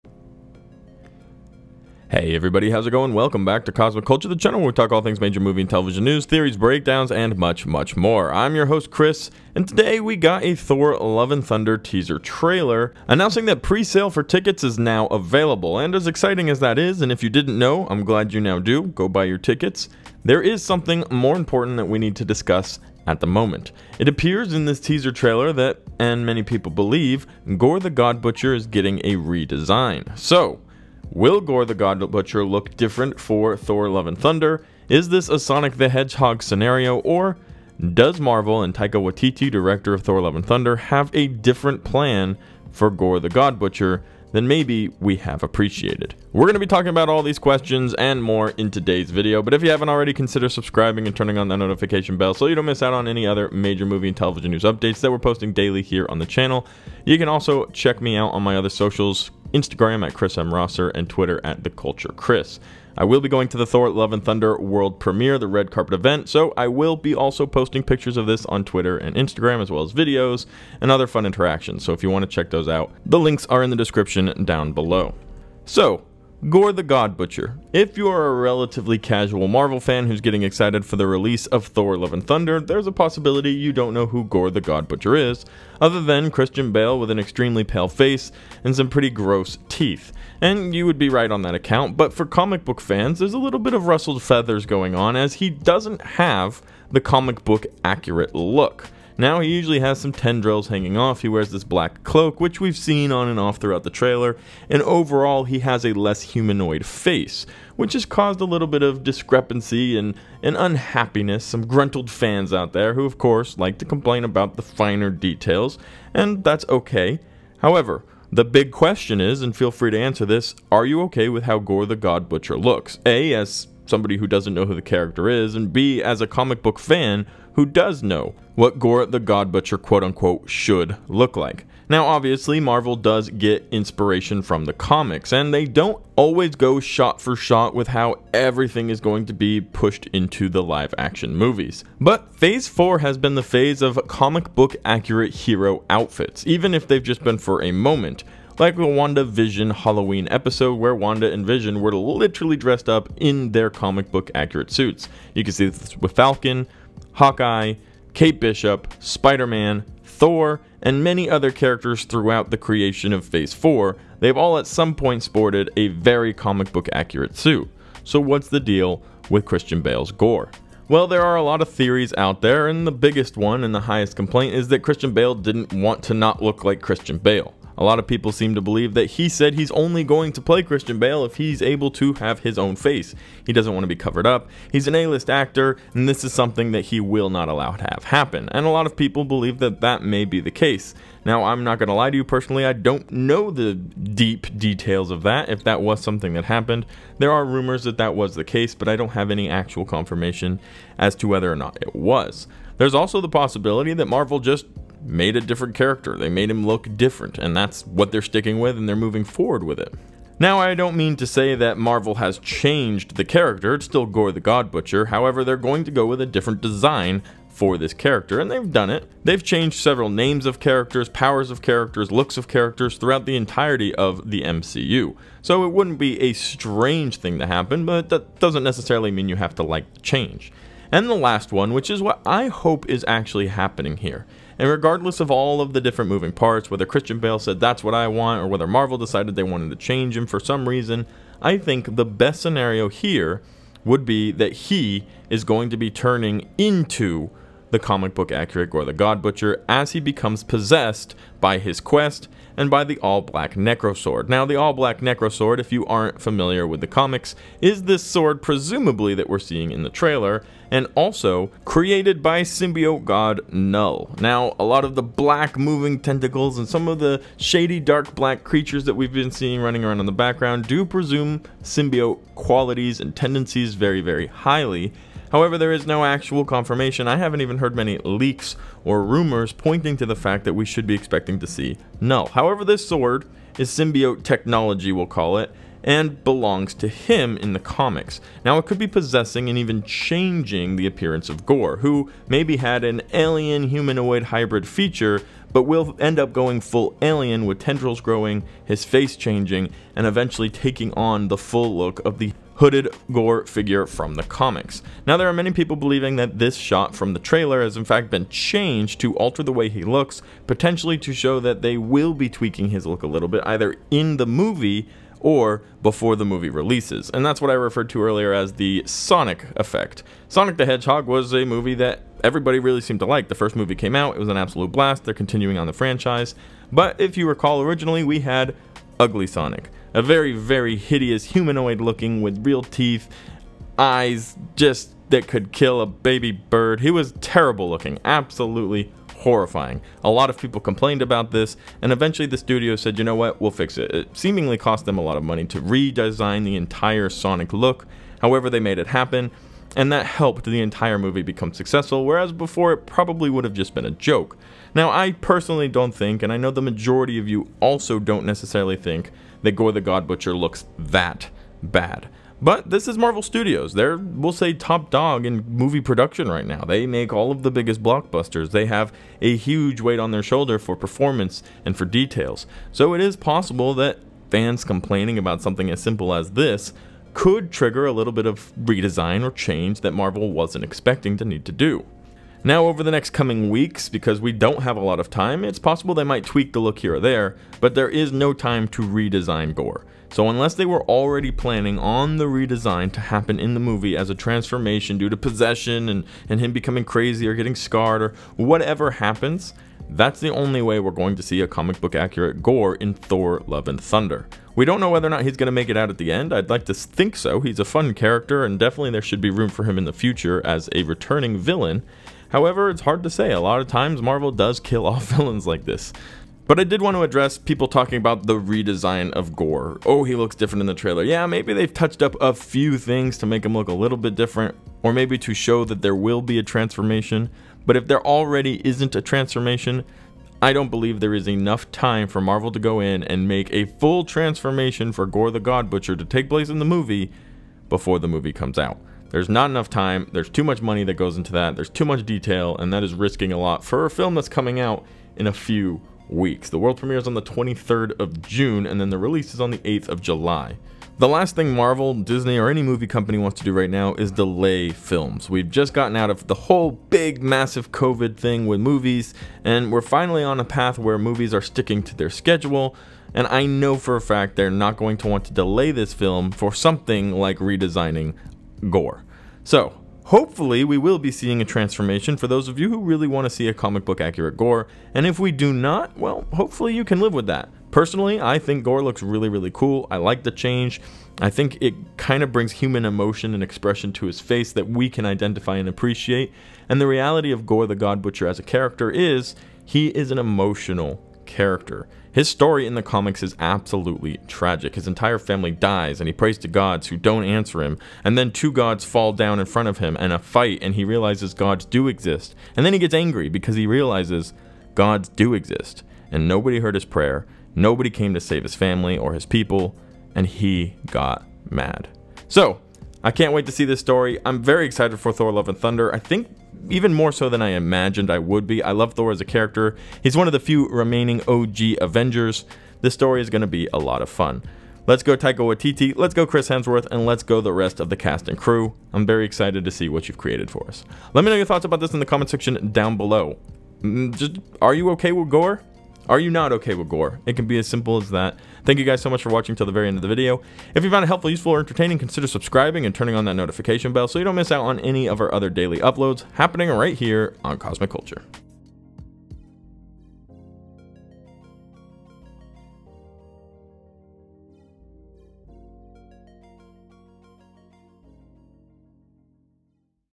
Thor Love And Thunder - Was Gorr The God Butcher REDESIGNED In New Trailer??
Lets talk about marvel, lets talk about thor love and thunder, lets talk about Gorr the god butcher. the upcoming MCU feature film has a new villain and from the trailers it seems you either love him or you hate him?

Memberships are now availbe! Click the link or join button to check out the awesome perks!